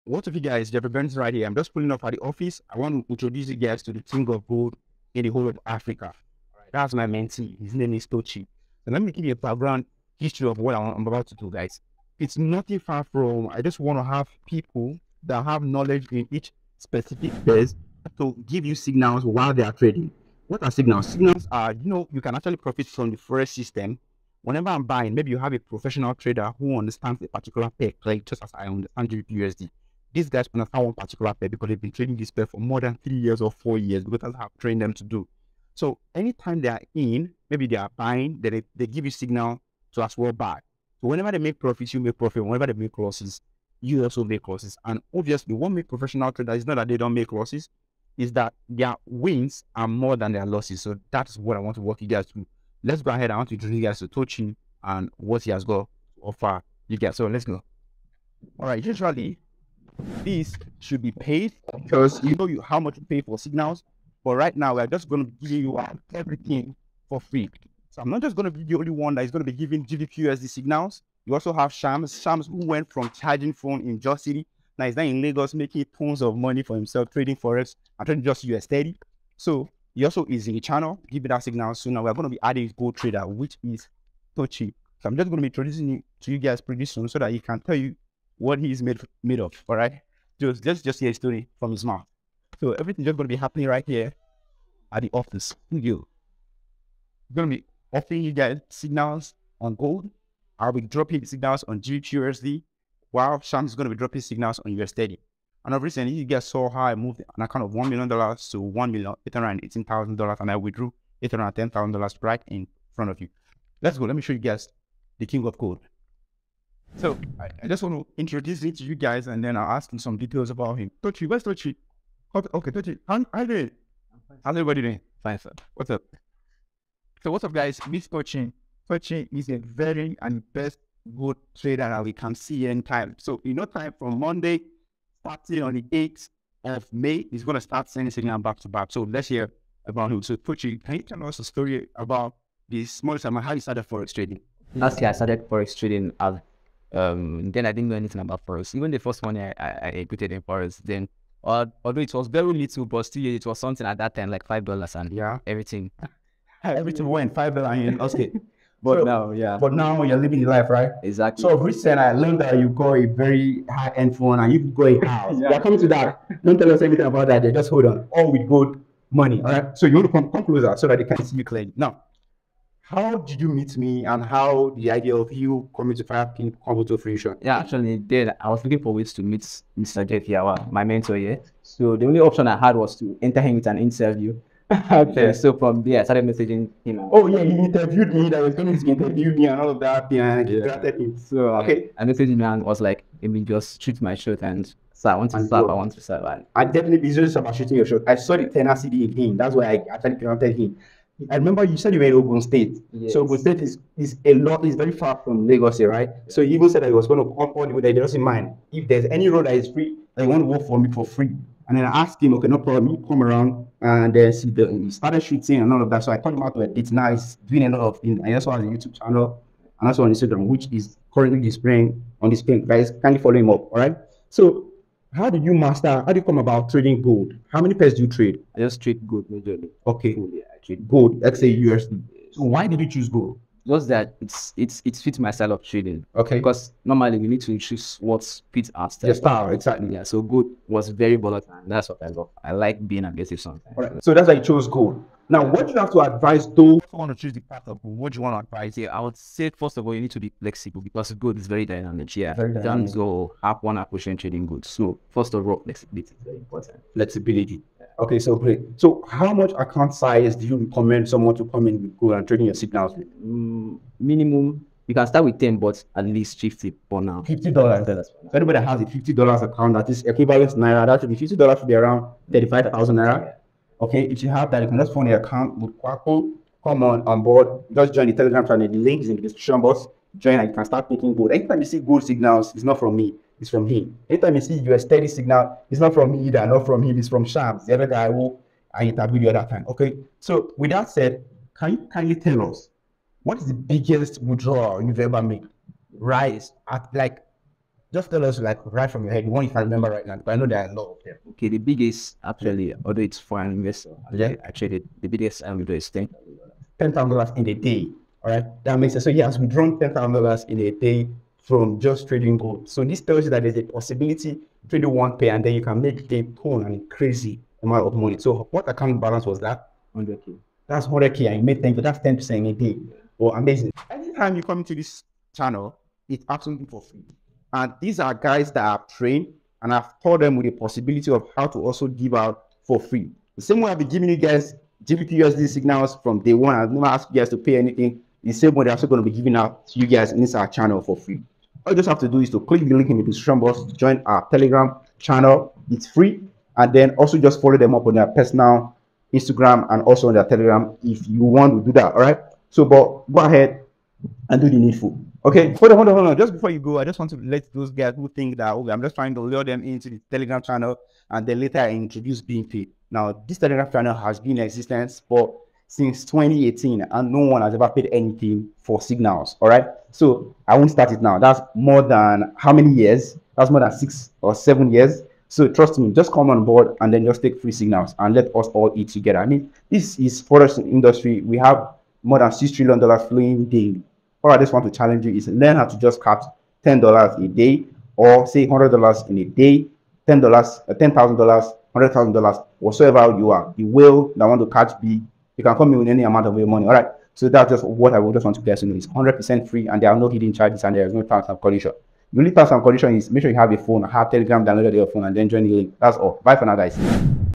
I Trained A Secret Gold (XAUUSD) Digger
Samuel Tochi is a Gold Day Trader in Lagos. And he has helped many pass their prop firm account with his Gold signals. And now, I'm bringing him to Firepip to unleash his secret strategy! ENJOY